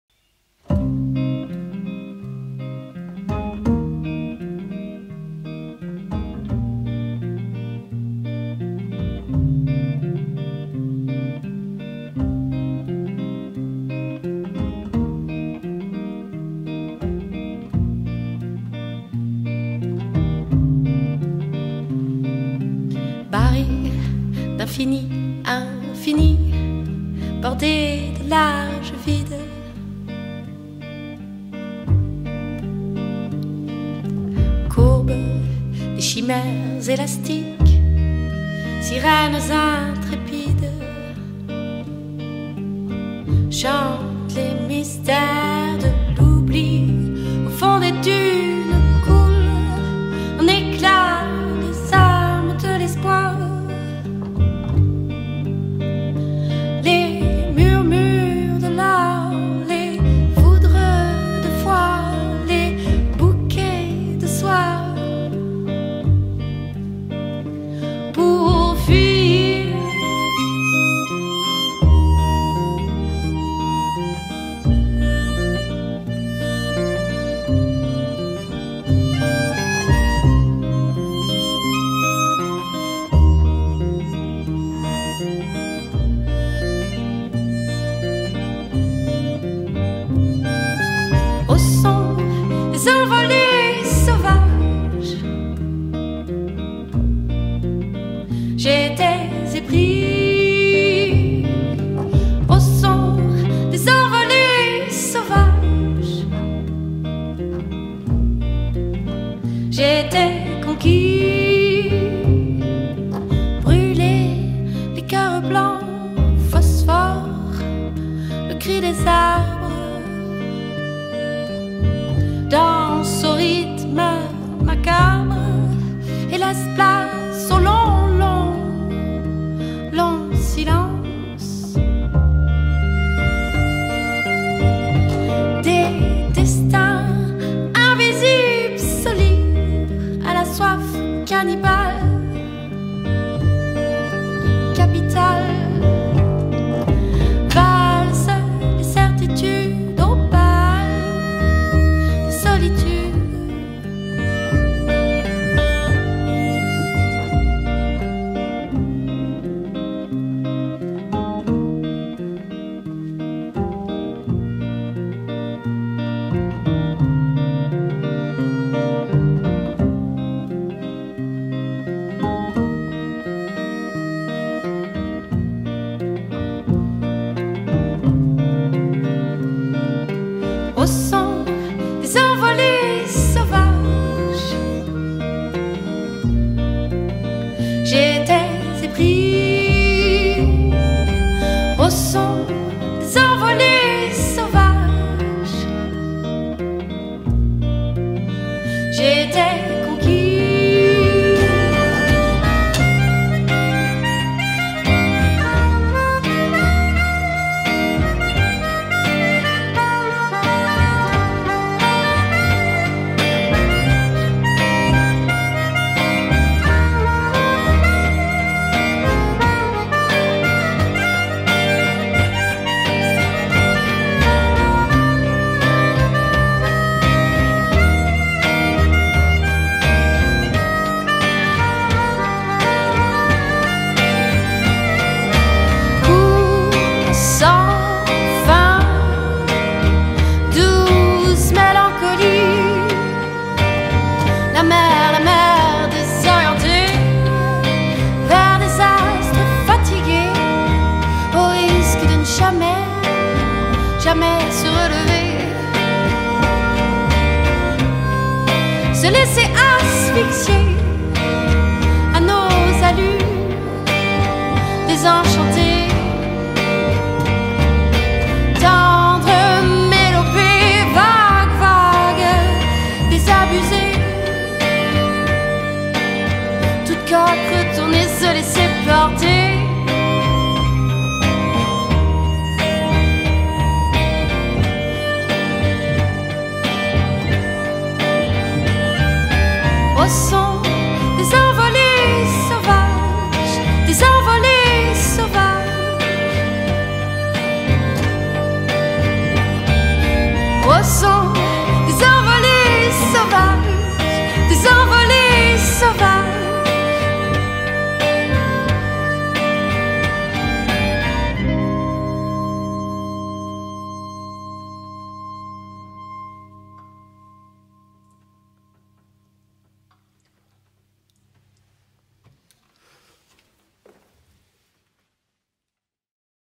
Les Envolées Sauvages live session
Invités au Prieuré de Grammont pour un live acoustique.